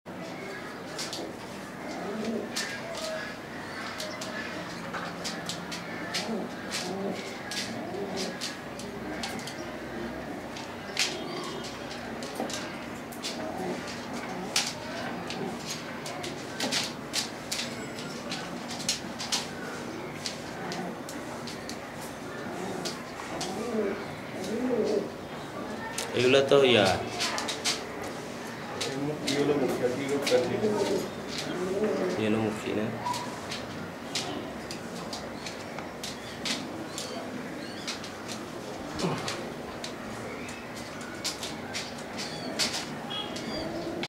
Worlds Expensive and Rare Yellow Mukkhi Loft or Pigeon
Yellow Mukkhi is very expensive for its rare collection and difficult breeding. Black Mukkhi is available in market and comparatively cheap in price.